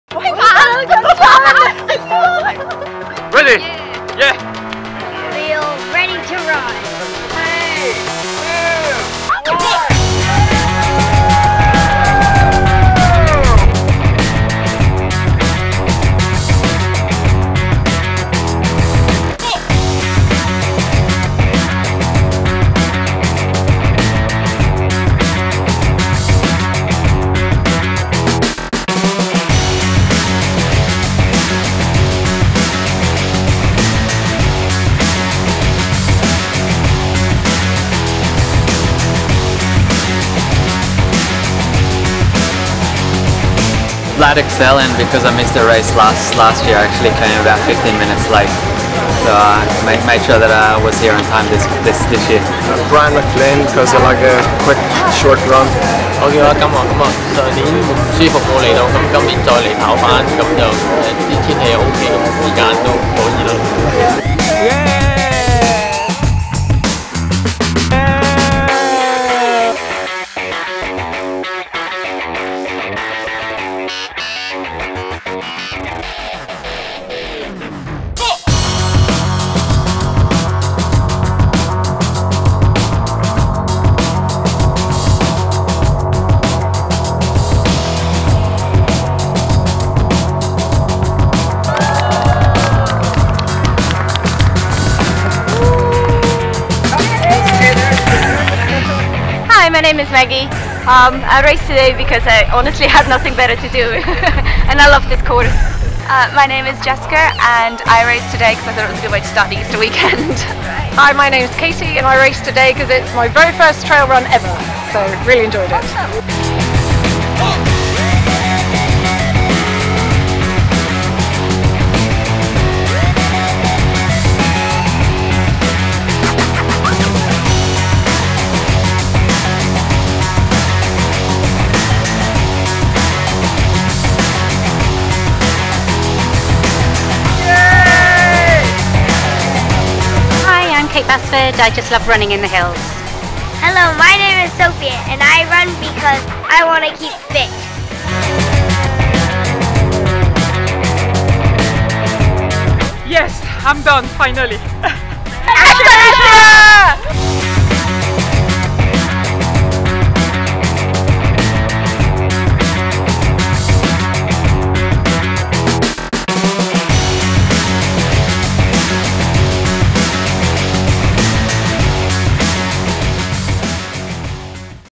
Inspiration - C3fit Bonaqua Action Sprint Repulse Bay
Inspirational video on the C3fit Bonaqua Action Sprint trail series in Hong Kong with race 1 at Repulse Bay Apr 14 2017...check out our trail series